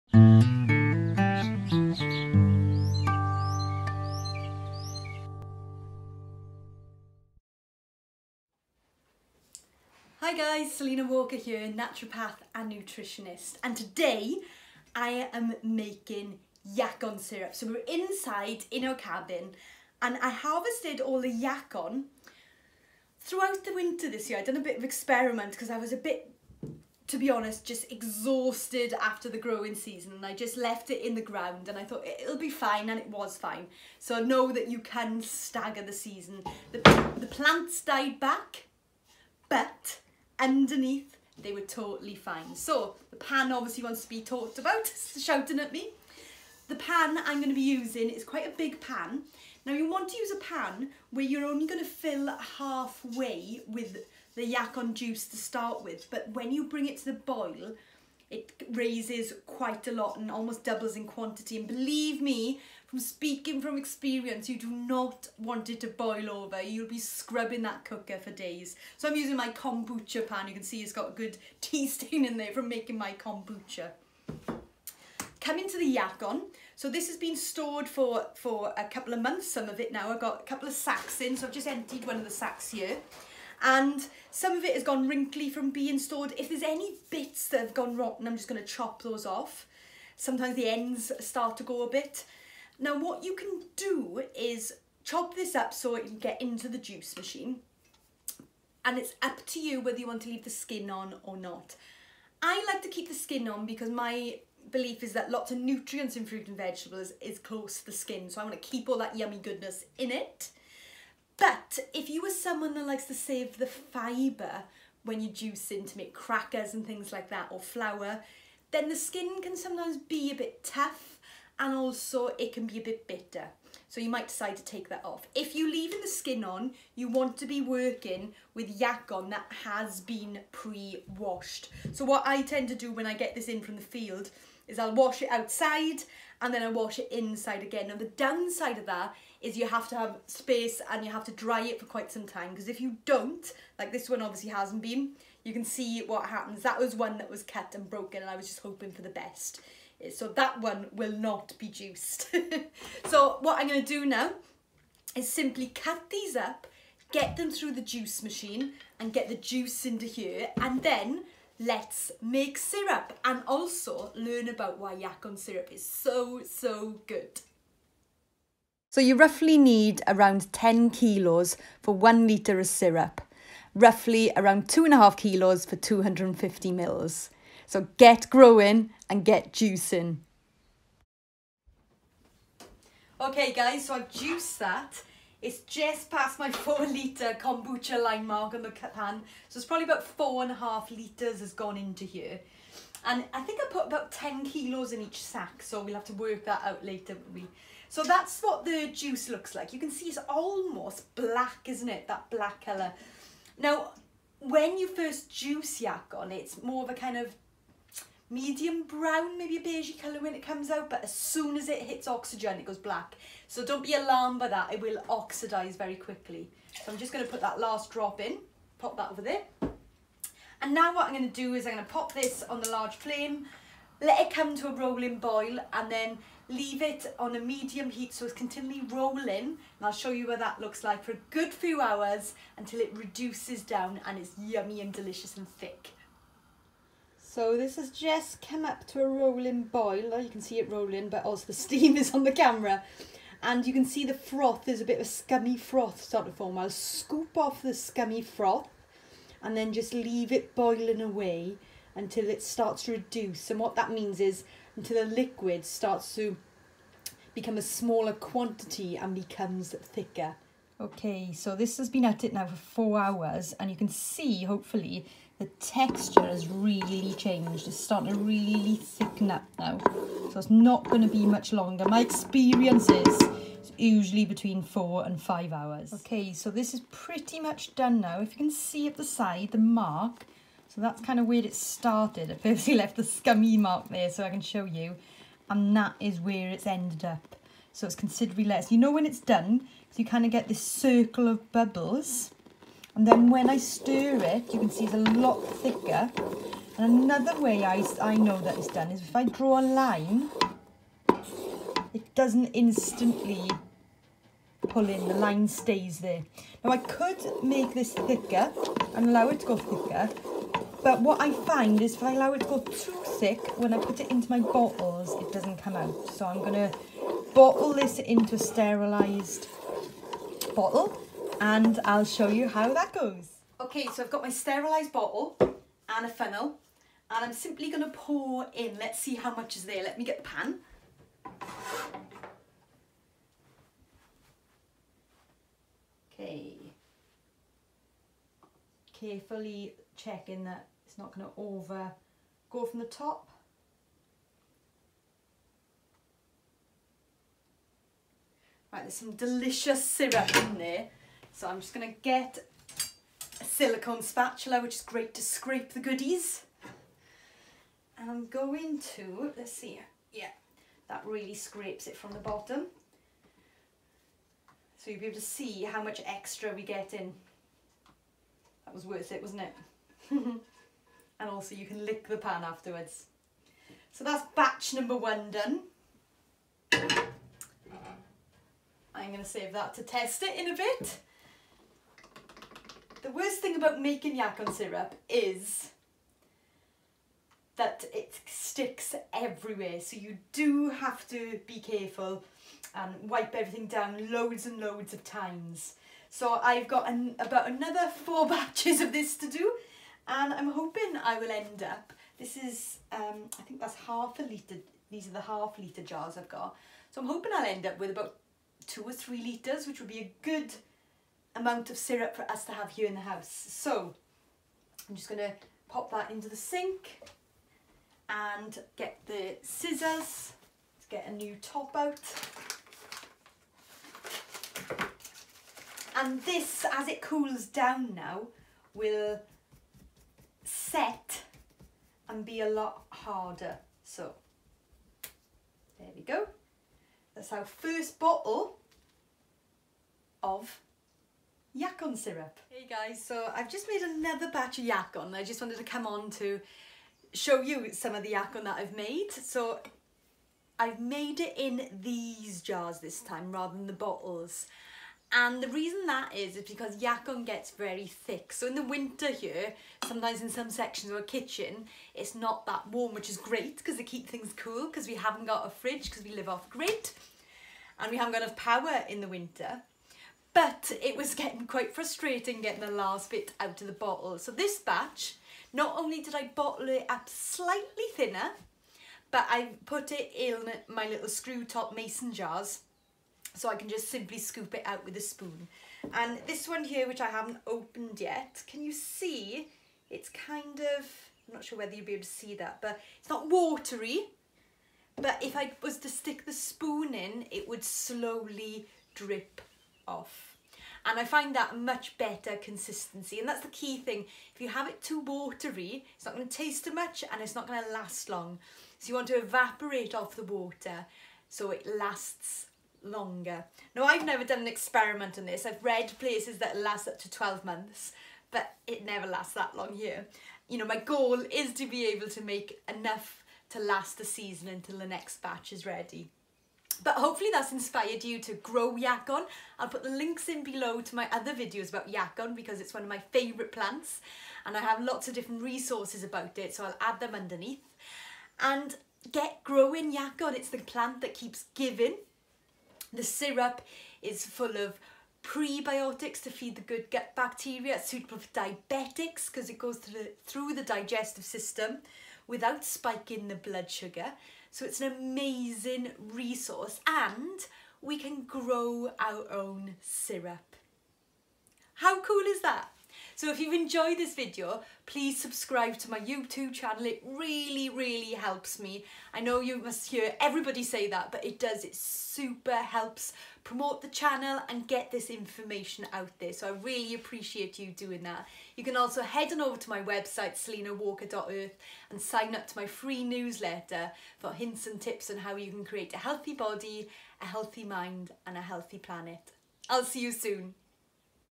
How To Make Yacon Syrup
You can grow your own syrup. Yacon has so many uses but one of the main reasons I grow it is to make my own syrup. The syrup is rich and dark and it’s brimming with nutrients. Clear, yellow syrups means that it has been refined and most of the nutrients have disappeared. As well as being rich in colour it is also rich in flavour, it tastes like malty caramel. It works extremely well in raw chocolates, in a mug of cacao, on buckwheat pancakes, in a chocolate congee and I also use it in many deserts.

For more hints and tips on how you can create a healthy body, healthy mind and healthy planet plus get some freebies sign up to my FREE newsletter